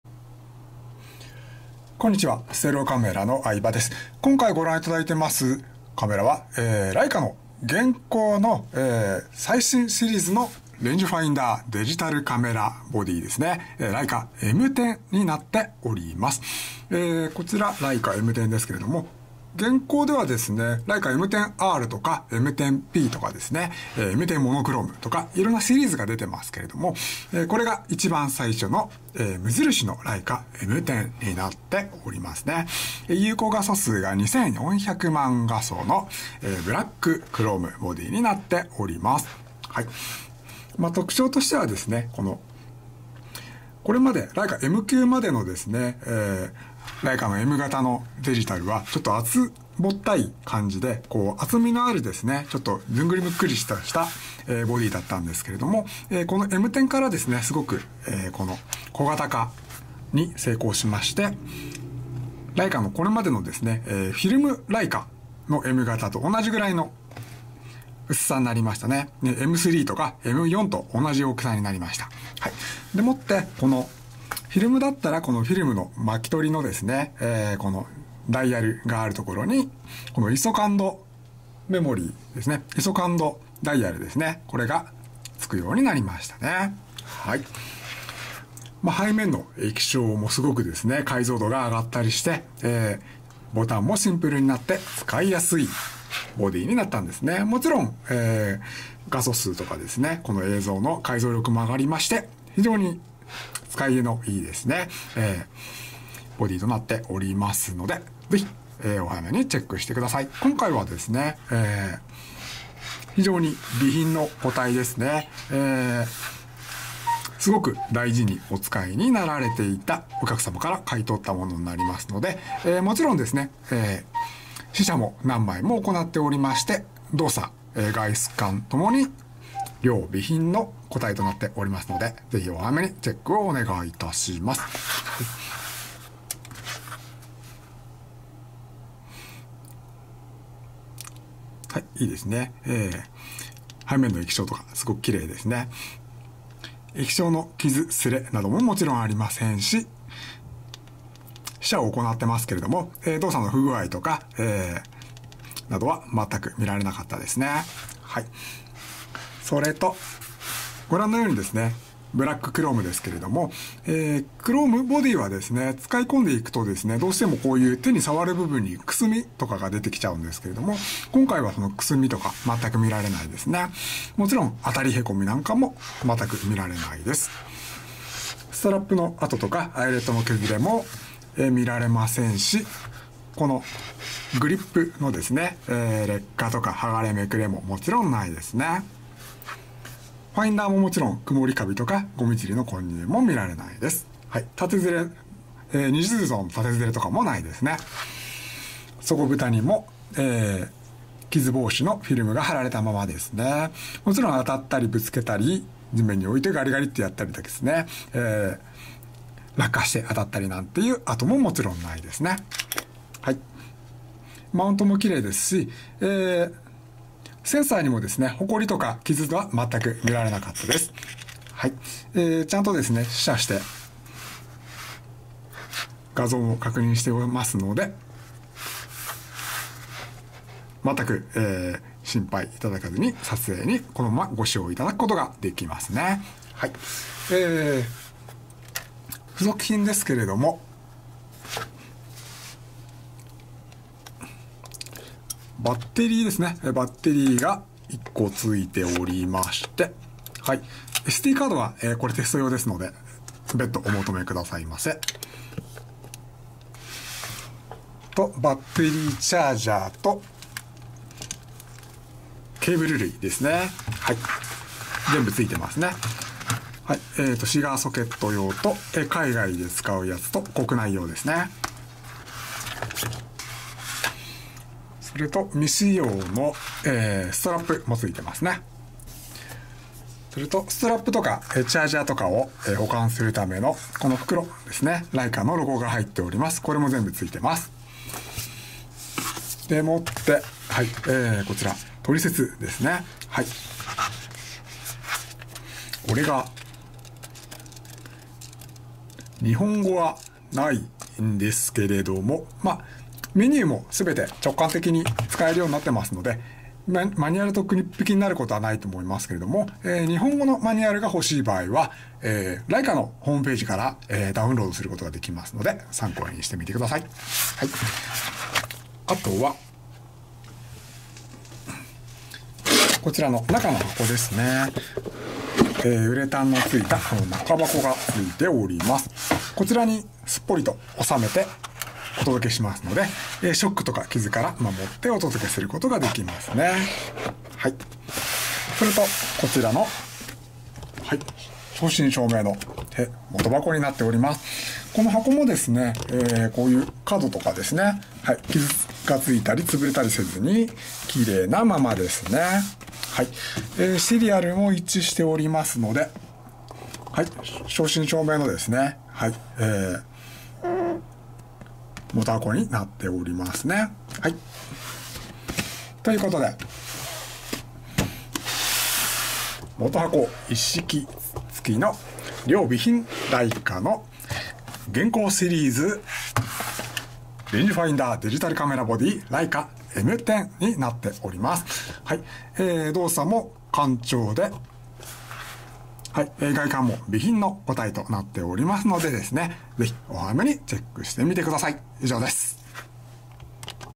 LEICA ライカ M10 Type3656 デジタル ブラッククローム 元箱一式
価格もこなれて、現在最おすすめのM型デジタル機
良美品。元箱一式付属。